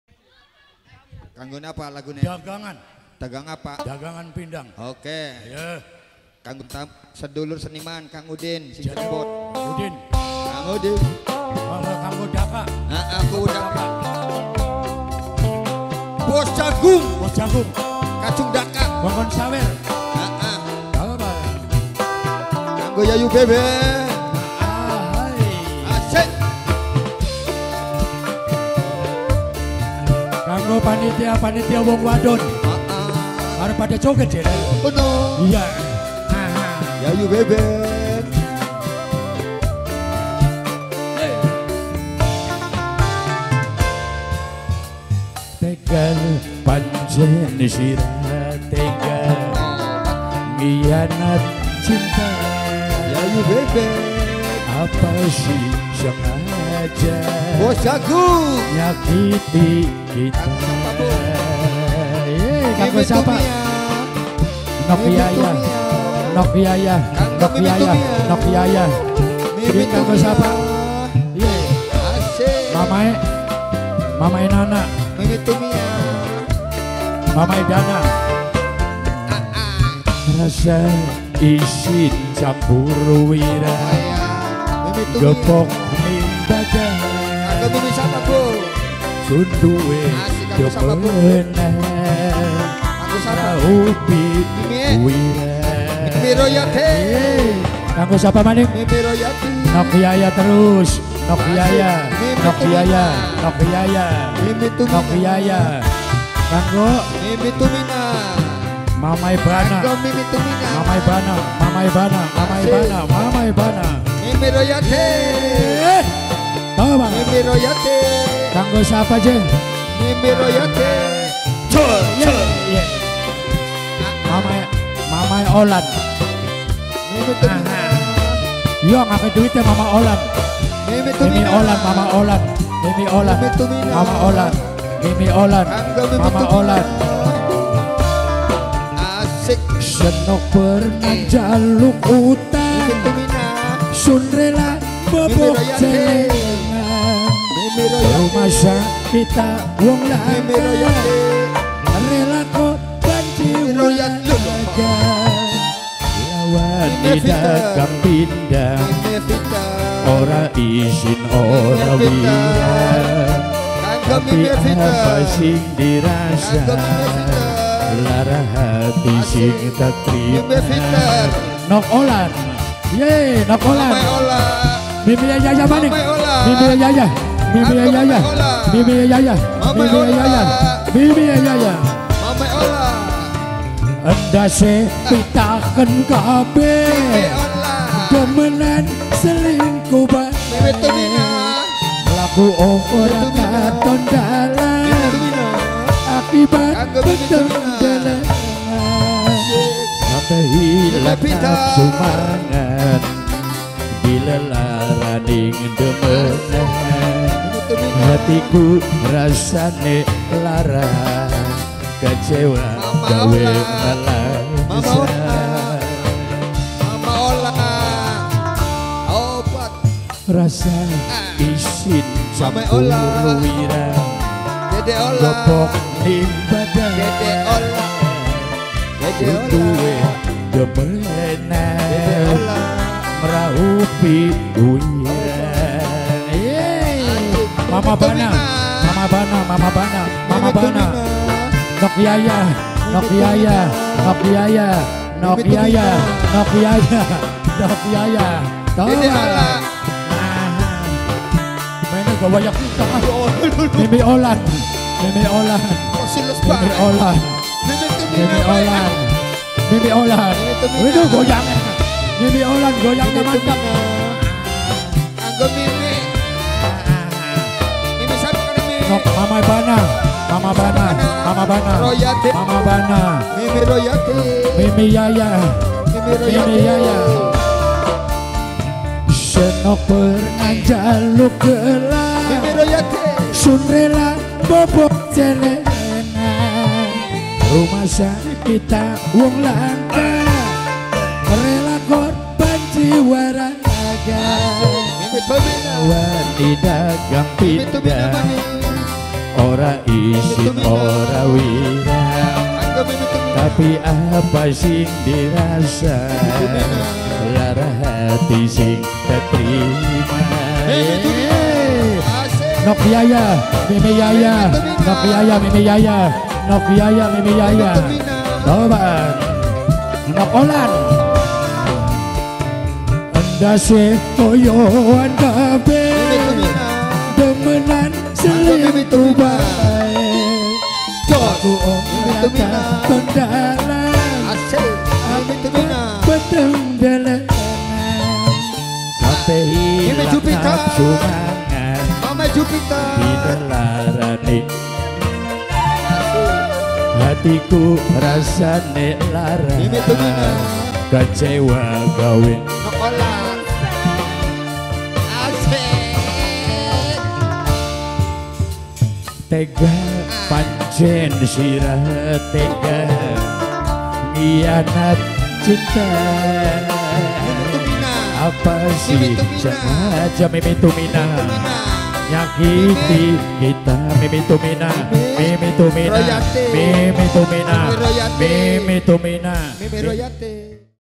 DAGANG PINDANG [SANDIWARA CHANDRA SARI]
DALAM RANGKA PESTA ADAT SEDEKAH DARAT ( UNJUNGAN ) BUYUT WANASARI TAHUN 2020 BERTEMPAT DI TPU BUYUT BANGKONG DESA WANASARI Kec. Bangodua - Indramayu JUM'AT 16 OKTOBER 2020